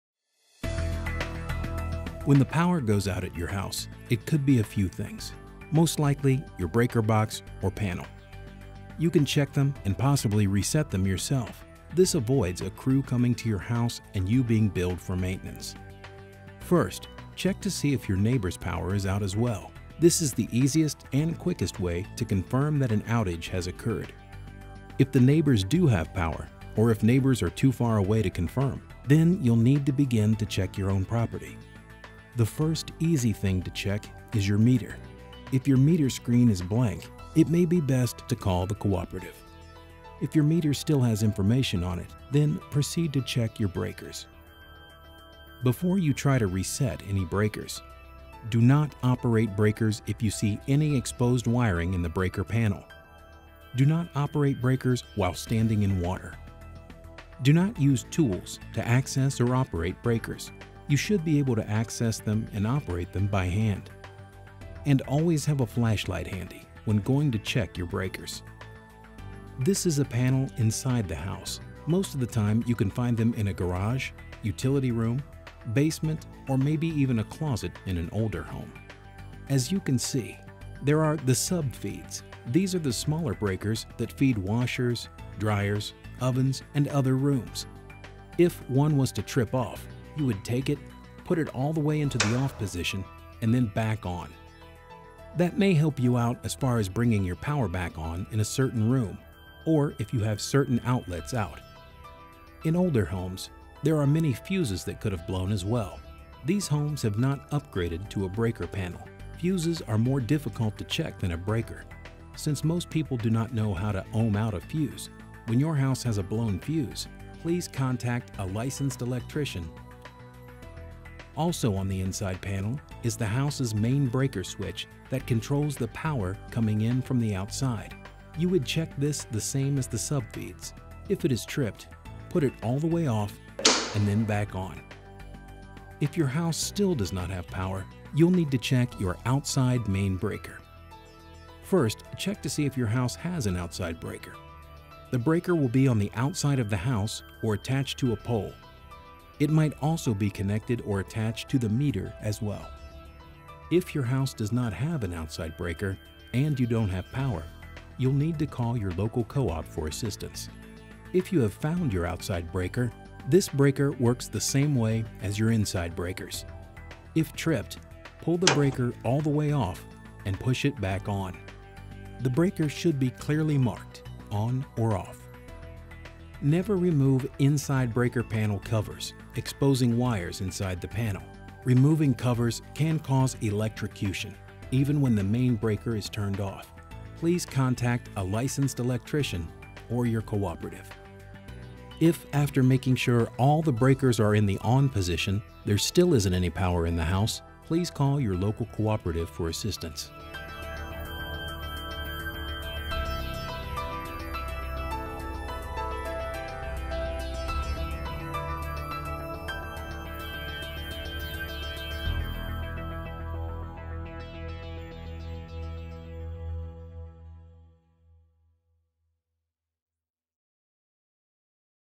2019 Check Your Main Breakers Instructional Video
Before calling your cooperative, you can check your outside and inside breakers.  This will avoid a crew coming to your house and you being billed for maintenance.  Here is a video to show what you can do with your breakers.